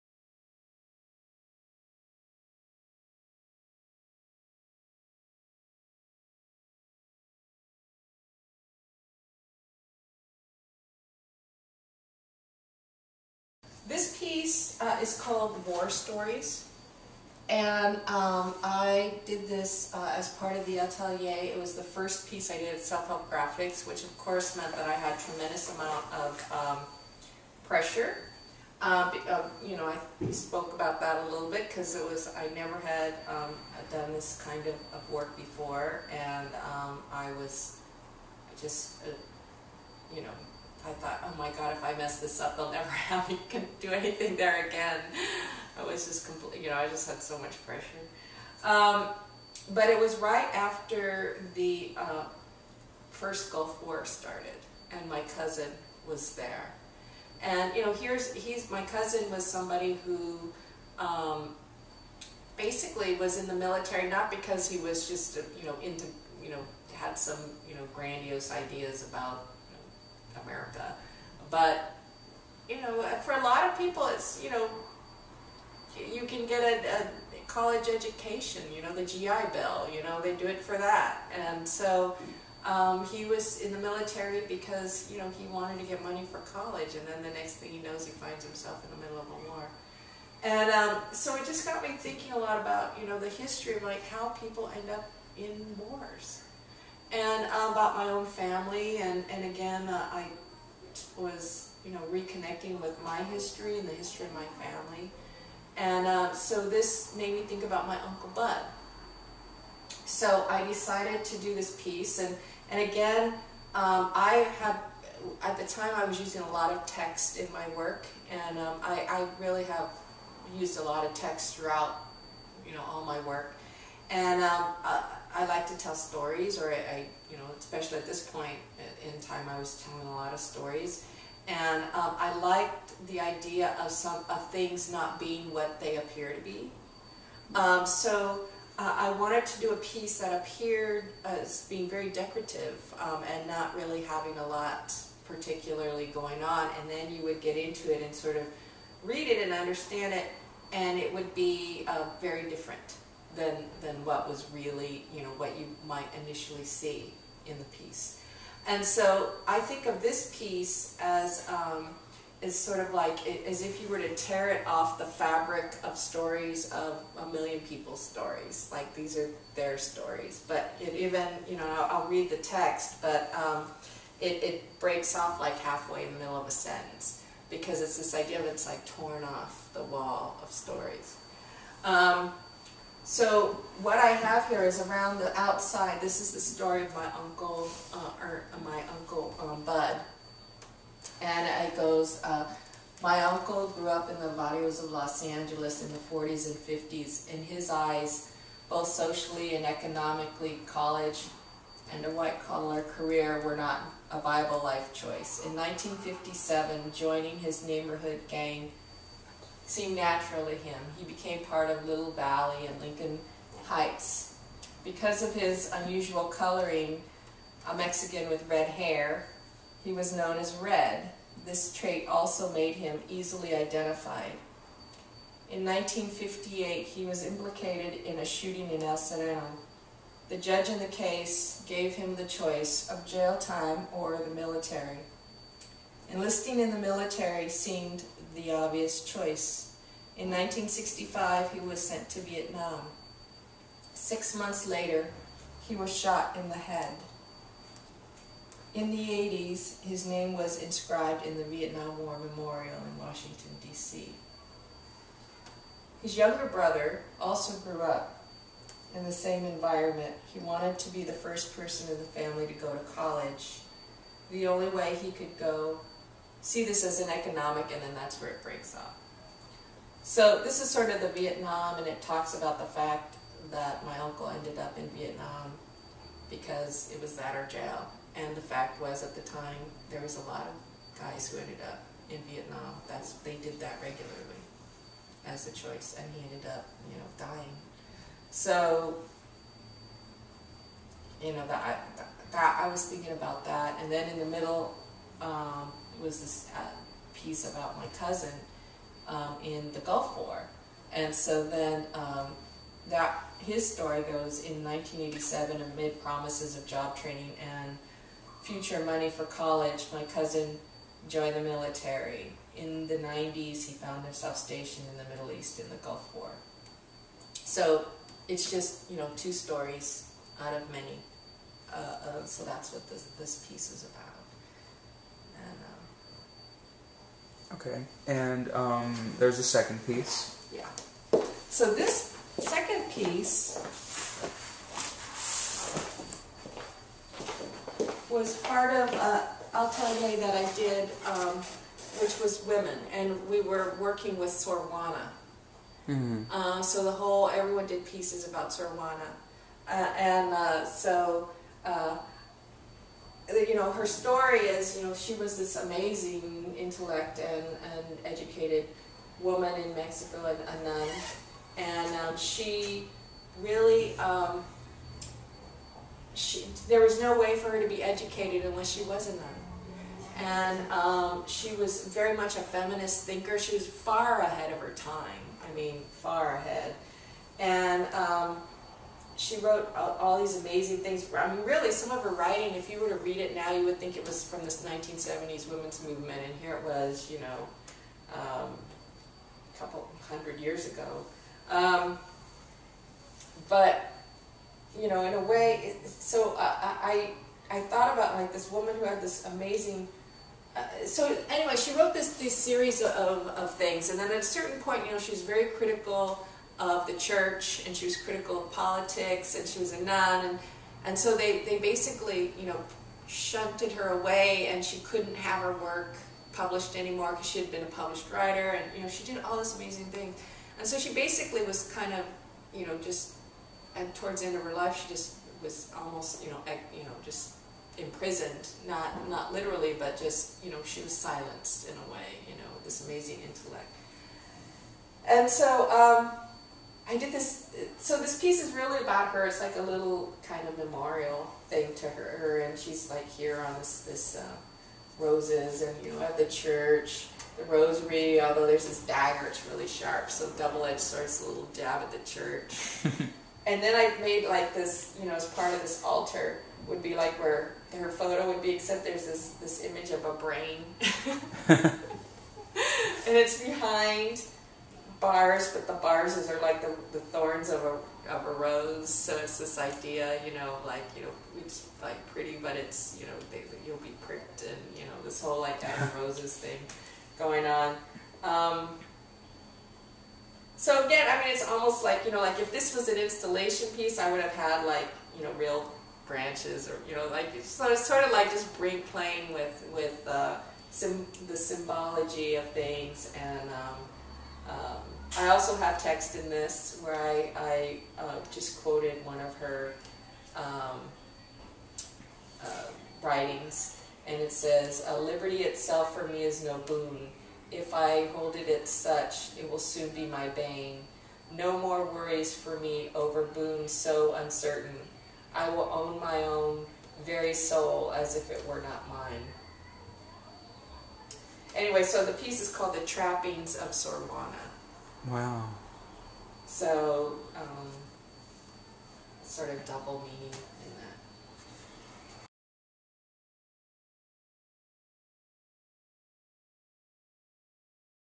Pat Gomez on "War Stories" & "The Trappings of Sor Juana"
Pat Gomez, graphic/installation artist, explains her art pieces "War Stories" & "The Trappings of Sor Juana."  This interview was conducted as part of a series of Oral Histories for the "Self-Help Graphics & Sister Karen Boccalero History Project" by the Studio for Southern California History in collaboration with Cal State LA Professor Dr. Angela Vergara.